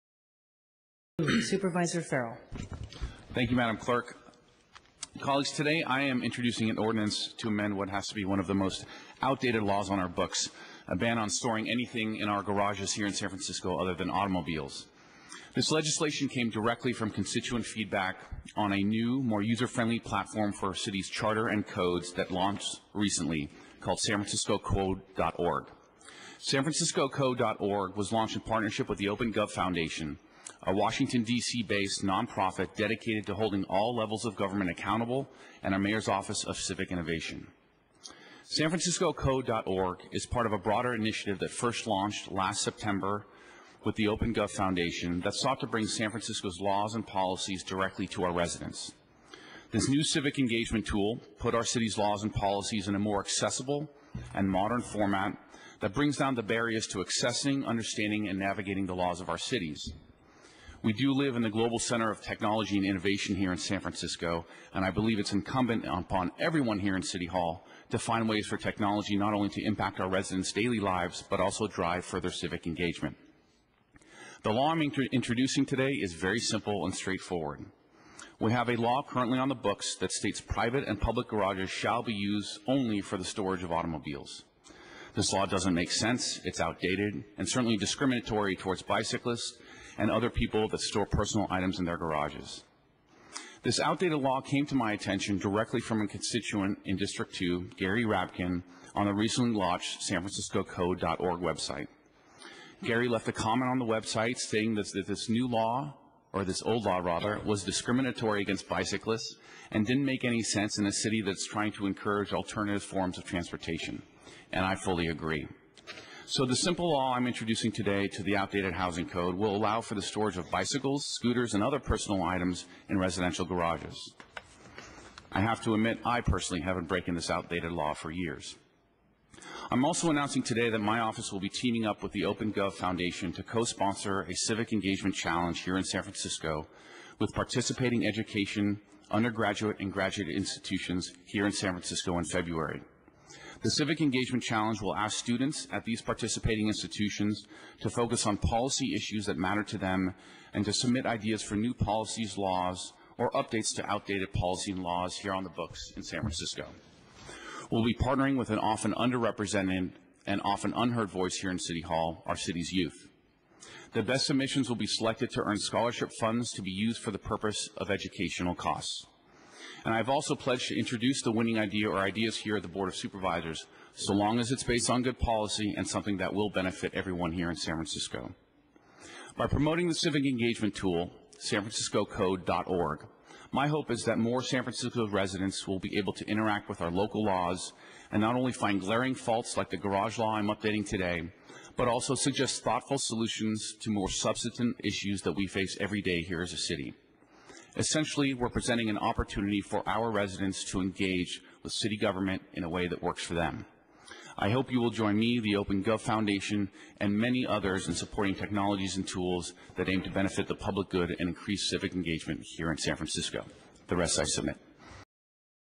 Building Better San Francisco Laws On the Internet with SanFranciscoCode org
San Francisco Supervisor Mark Farrell introduces a House reform ordinance generated by one of his District 2 constituents - Gary Rabkin - flagging an out-of-date section of the city's municipal code with the new It's the first edition of the laws for everyday people living in the Internet Age.

1-7-13 from the San Francisco Supervisors' Meeting